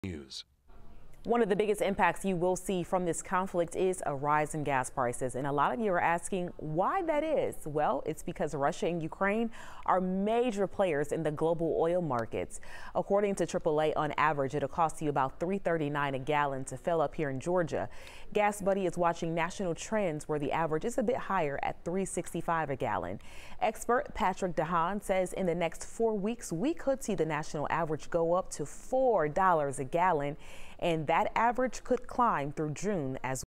Rise in gas prices across the nation amid Russia and Ukraine conflict
Russia and Ukraine are major players in the global oil markets.